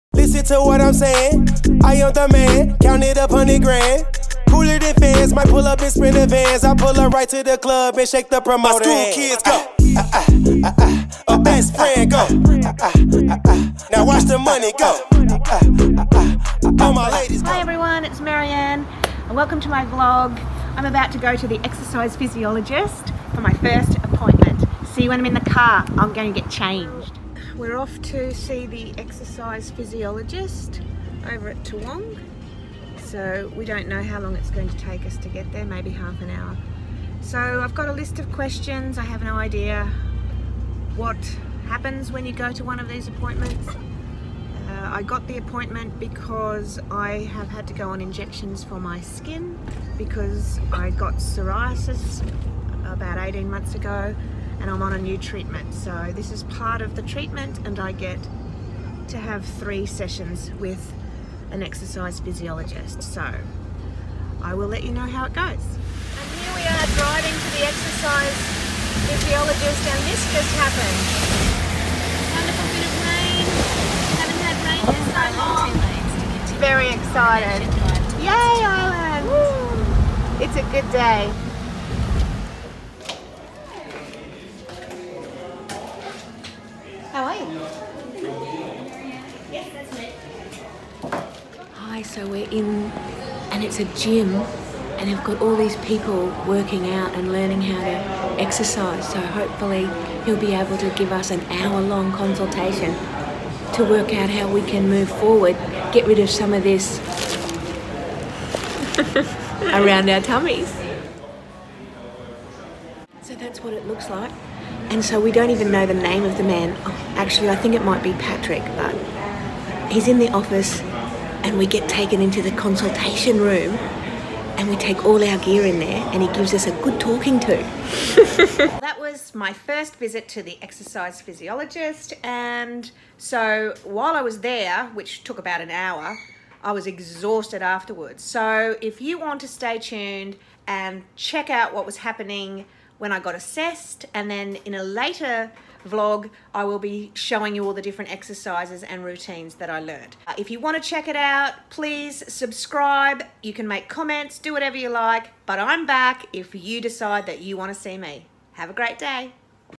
First visit to the exercise physiologist part 1
This is our first vlog. This is part one of MaryAnn visiting Body Track exercise physiology in Toowong, Brisbane.